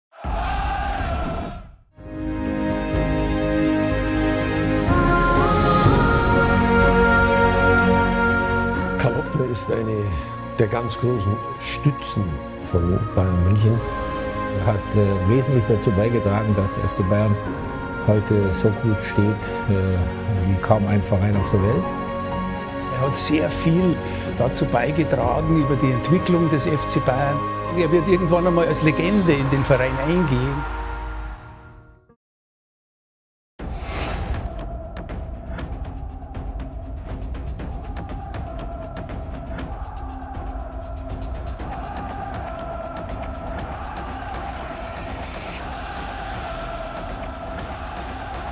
33 Jahre FC Bayern - Danke, Karl Hopfner!
Nach 33 Jahren verlässt unser Präsident Karl Hopfner den FC Bayern München. 1983 kam er als Geschäftsführer zum FCB und übernahm in seiner Zeit beim Rekordmeister diverse Ämter: Vorstandsmitglied, Aufsichtsratsvorsitzender, 1. Vizepräsident und schlussendlich Präsident. Kaum einer erwarb sich solch großer Verdienste um den Verein wie der 64-Jährige. Uns bleibt daher nur noch eins zu sagen: Karl, vielen Dank für alles, was du für den FC Bayern geleistet hast und alles Gute für deine Zukunft